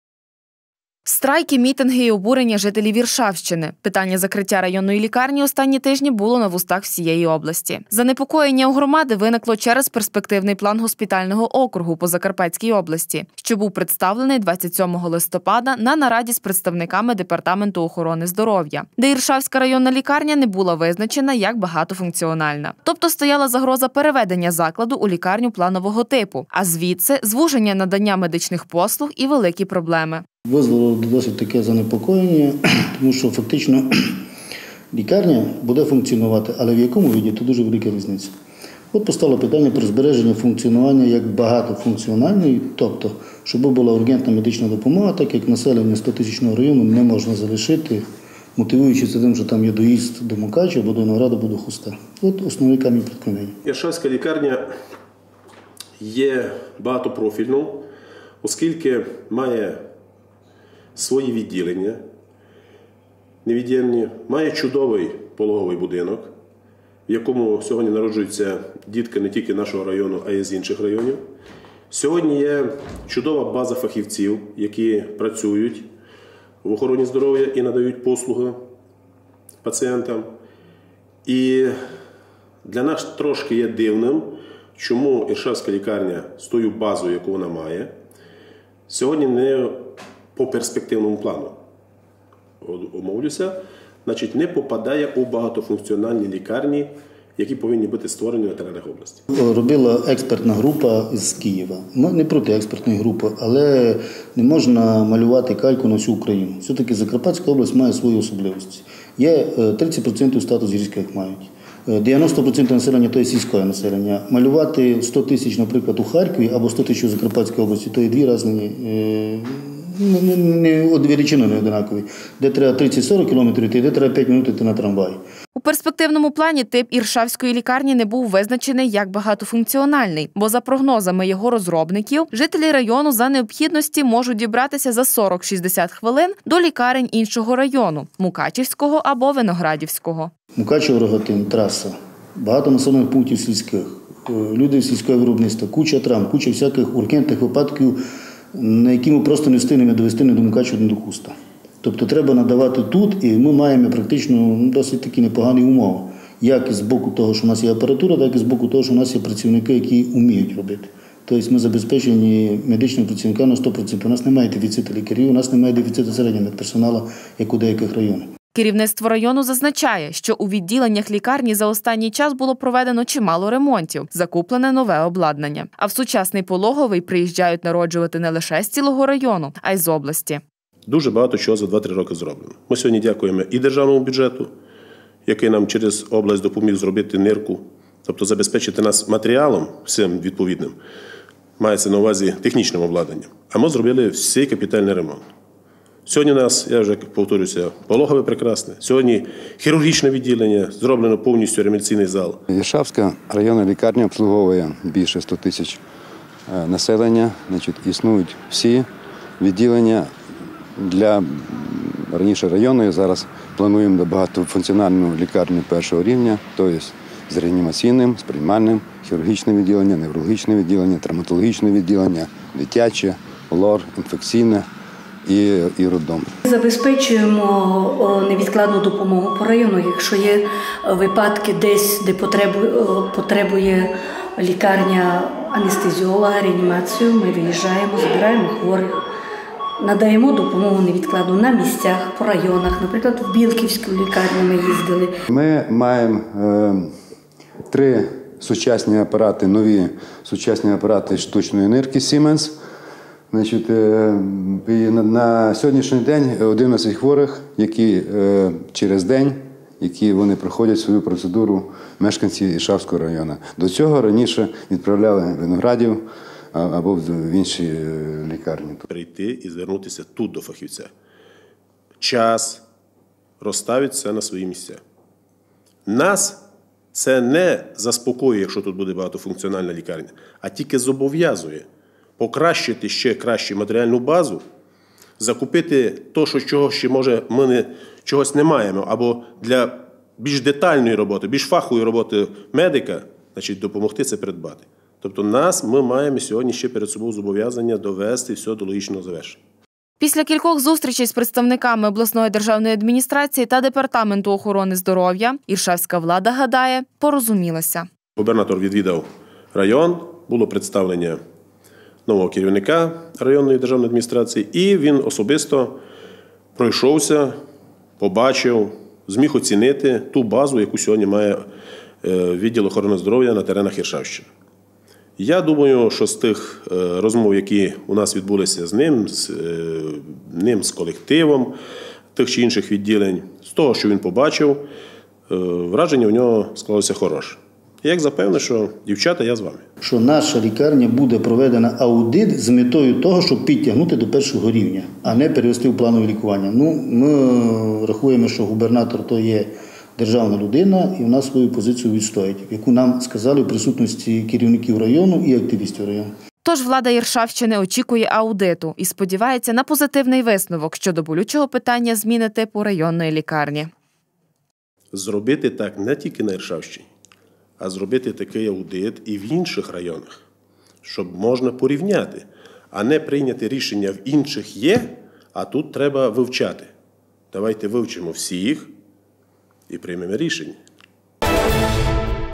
Питання лікарні
Страйки, мітинги і обурення жителів Іршавщини: питання закриття районної лікарні в останні тижні було на вустах усієї області. Занепокоєння громади виникло через перспективний план госпітального округу по Закарпатській області, що був представлений 27 листопада на нараді з представниками департаменту охорони здоров’я. Тепер, після кількох зустрічей із представниками ОДА та департаменту охорони здоров’я іршавська влада вважає: порозумілися. Тож там очікують аудиту і сподіваються на позитивний висновок щодо болючого питання.